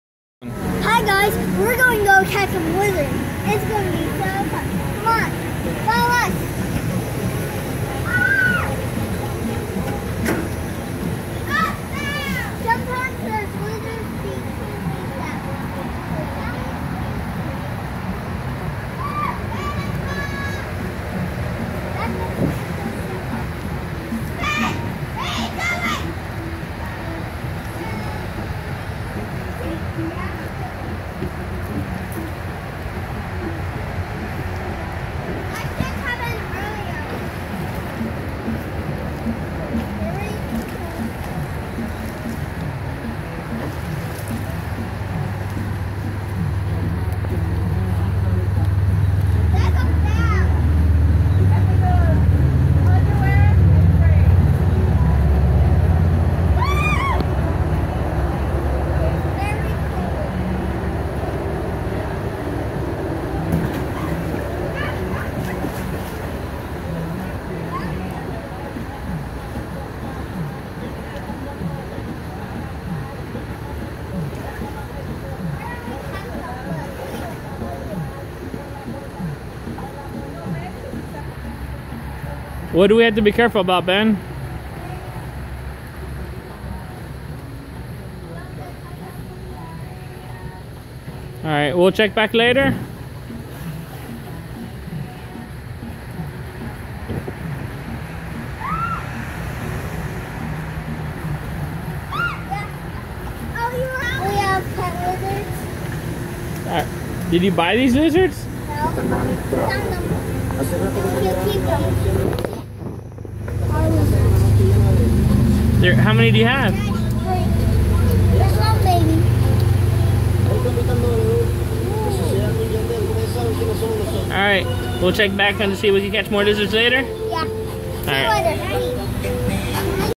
Ben going on a Lizard hunt
At Tio and Tia's house, and Ben saw some lizards he wanted to try to catch.